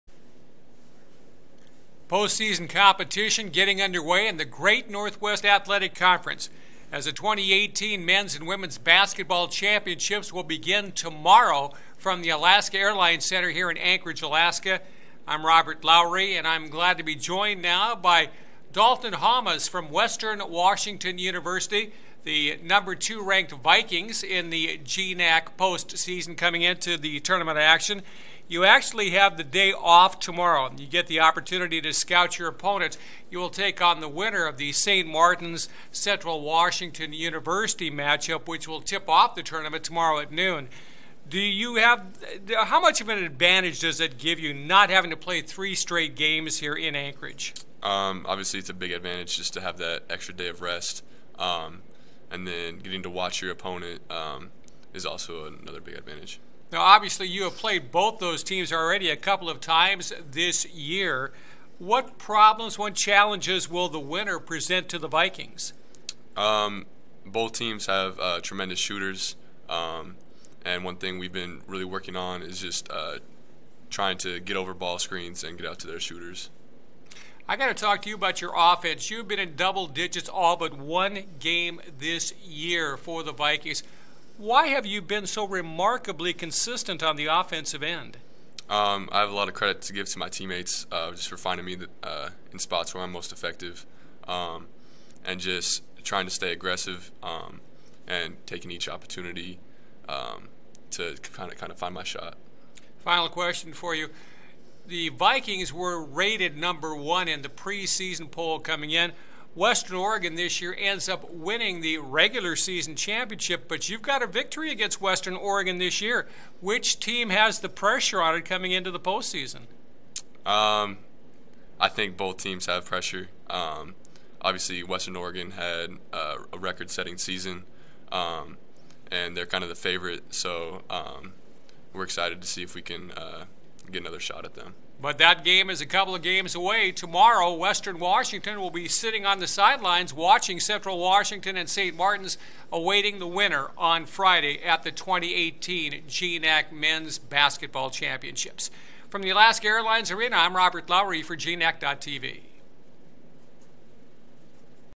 GNAC Basketball Championships - Daulton Hommes
Robert Lowery of GNAC.tv interviews Western Washington's Daulton Hommes on practice day of the GNAC Basketball Championships on Feb. 28, 2018